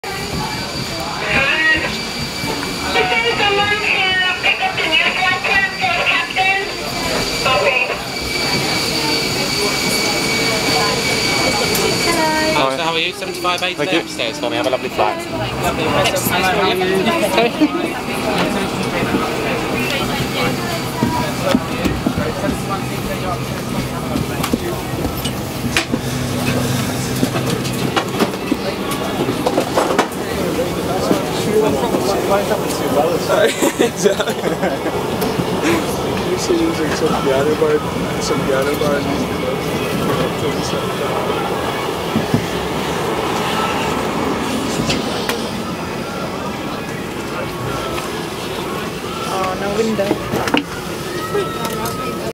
Virgin Atlantic 747-400 Premium Economy. Las Vegas - London Gatwick.
Boarding a Virgin Atlantic Boeing 747 at Las Vegas. Heading back to London Gatwick on "Ladybird".

While it was comfortable, it is worth noting that some of the fleet ("Virginia Plain" was the same on the outbound flight), appear to have the old style seating, unlike the Heathrow planes. Not that I was all that bothered either way because I just wanted legroom!!! I heard other passengers who have obviously used Virgin from Heathrow before, sounding shocked that the interior hadn't been updated yet. Admittedly I noticed that the seats looked very worn, the entertainment system was ancient and I think the surround of the TV on the back of my seat had even fallen off!!!

This video clip is for the benefit of anyone considering booking a Virgin flight from Gatwick. Hope it helps. The camera was dangling down from my neck as I was boarding and walking to the seats, hence it being a bit unsteady.

I just wish I was recording at the end of the flight when someone on the PA system announced, "Cabin crew, doors to manual and cross dress"...

November 2011.